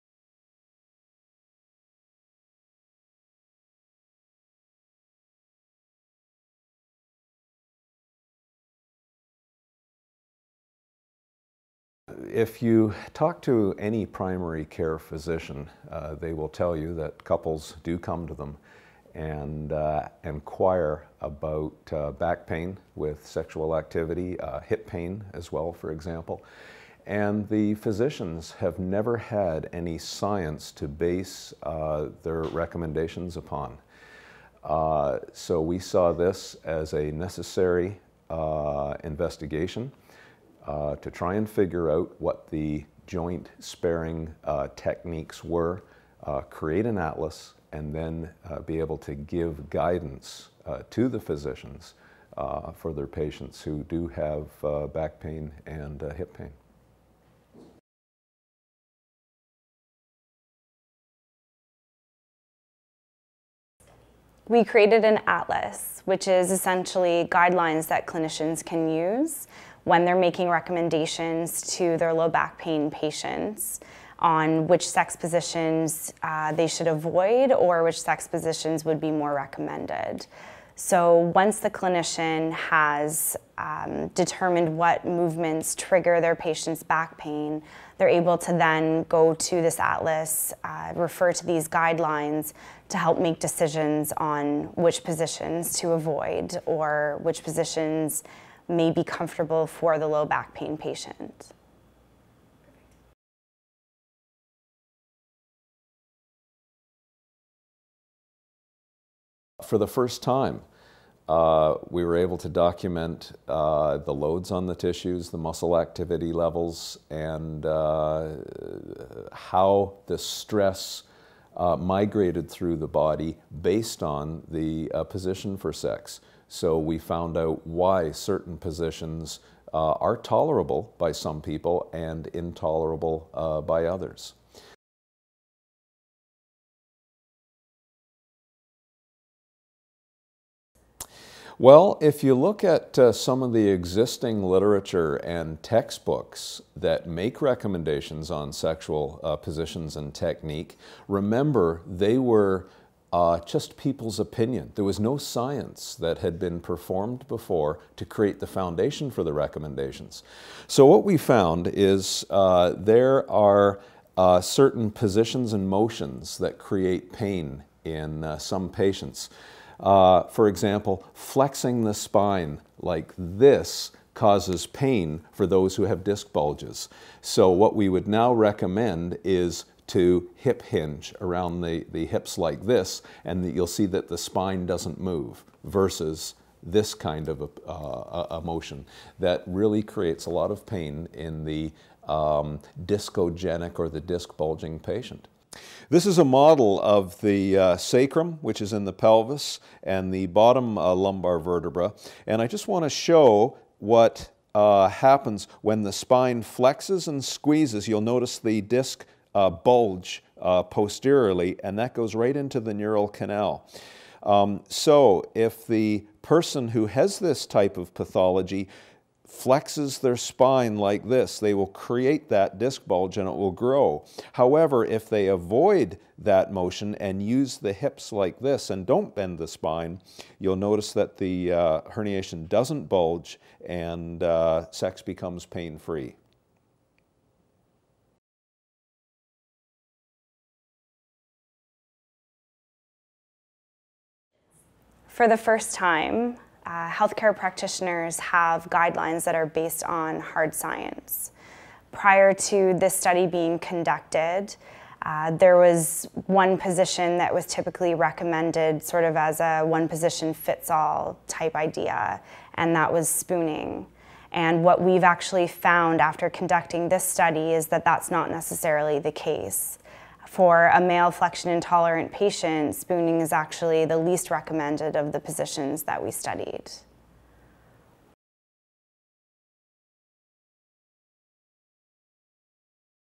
Groundbreaking study reveals best positions to save your spine during sex
Contrary to popular belief, spooning is not always the best sex position for those with a bad back, according to research from the University of Waterloo. For the first time ever, scientists have successfully documented the way the spine moves during sex and discovered exactly why certain positions are better than others when it comes to avoiding back pain.

The pioneering study combined infrared and electromagnetic motion capture systems – like those used in the creation of video games – to track how 10 couples’ spines moved when attempting five common sex positions. The findings were used to create an atlas, or set of guidelines, that recommends different sex positions and thrusting techniques based on what movements trigger a patient’s pain.

In this video, Waterloo’s Dr Spine – Professor Stu McGill – and the study’s lead author, Natalie Sidorkewicz explain how they carried out the research and what they found out.